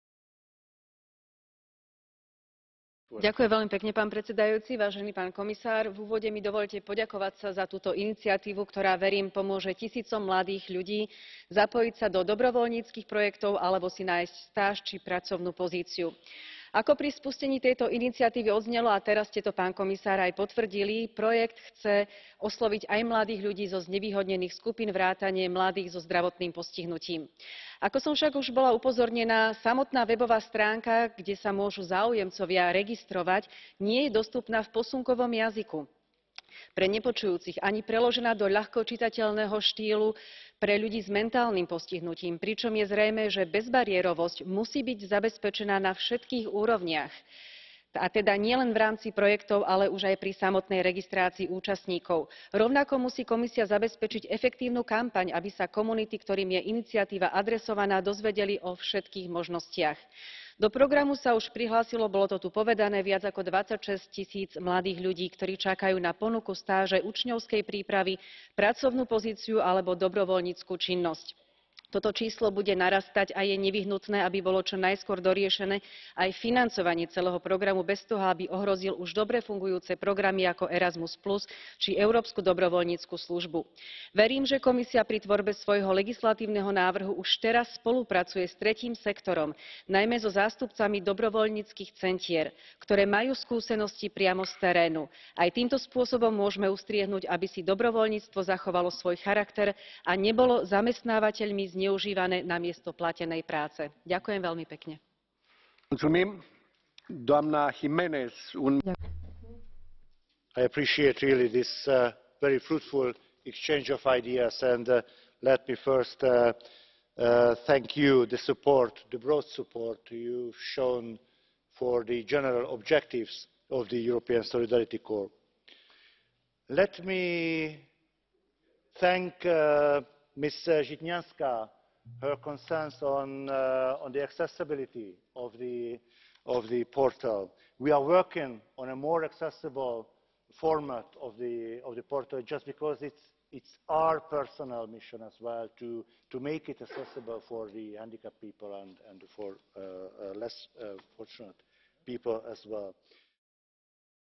Jana Žitňanská: Možnosti pre mladých so zdravotným postihnutím
Jednou z pozitívnych stránok nášho členstva v EÚ sú aj možnosti pre mladých zúčastniť sa na rôznych stážach, dobrovoľníckych projektoch, zísakť pracovnu príležitosť. V mojom vystúpení k Európskemu zboru solidarity som však upozornila, že nesmieme zabudnúť ani na mladých so zdravotným postihnutím a to už pri samotnej registrácii do programu. Tu je moje vystúpenie a následne reakcia komisára pre vzdelávanie, mládež a kultúru Navracsicsa.